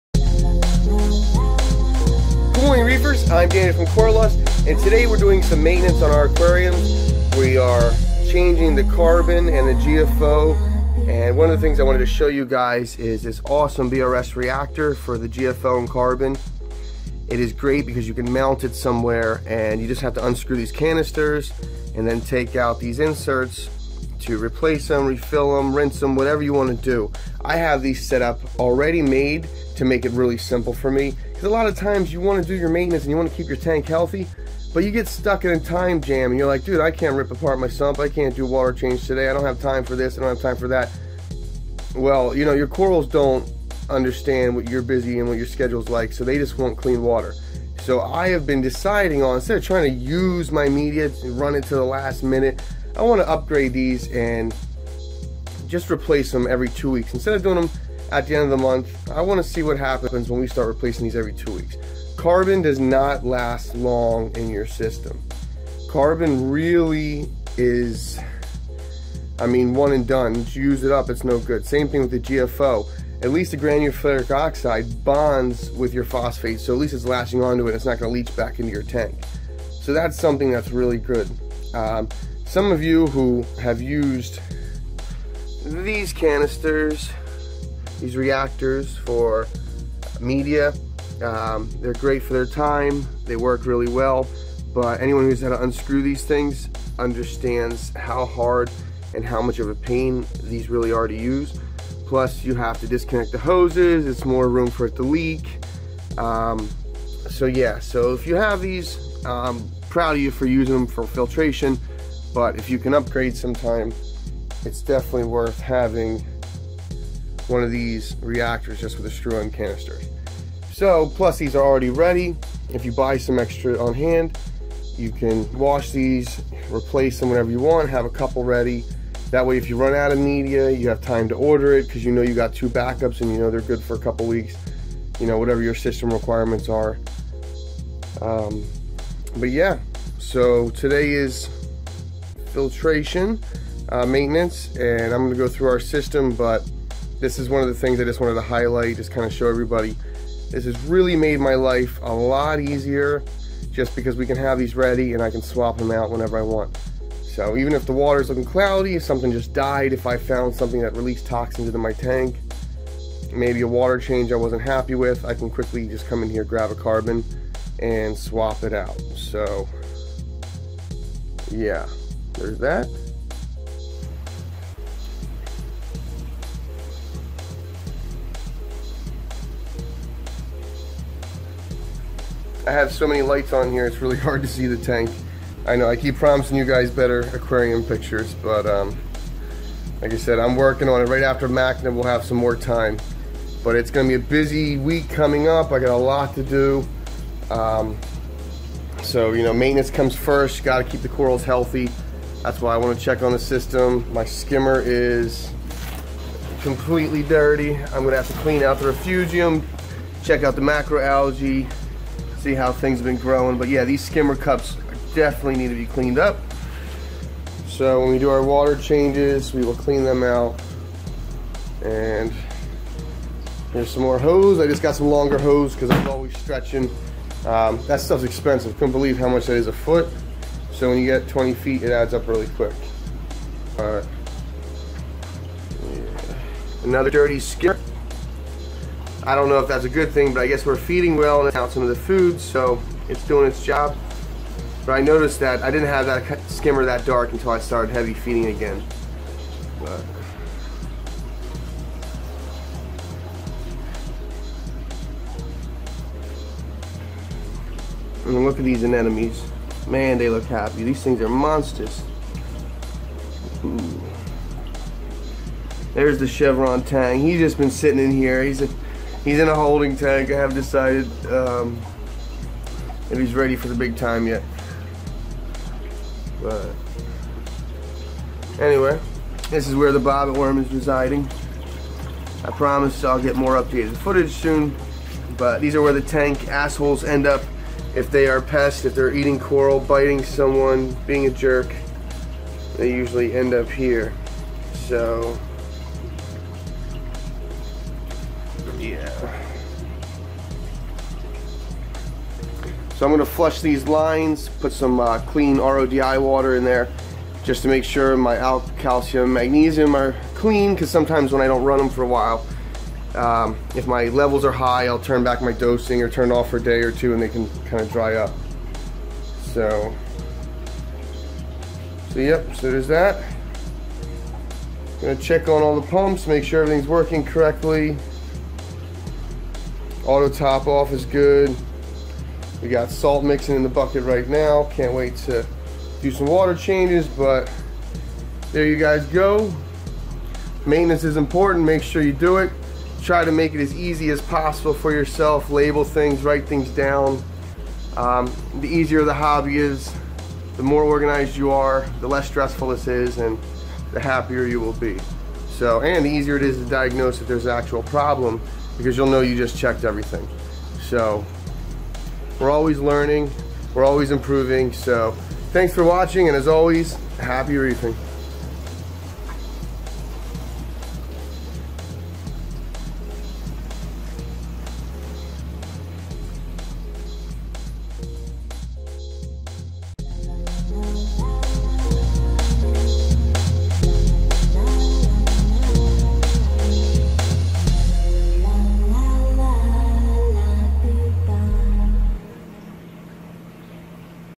Display and Frag Tank Filtration maintenance
We have been experimenting with our new carbon and GFO reactors trying to get the right amount of media for each tank and time the media replacement with water changes and skimmer cleaning to help maximize the media life span of the carbon and GFO.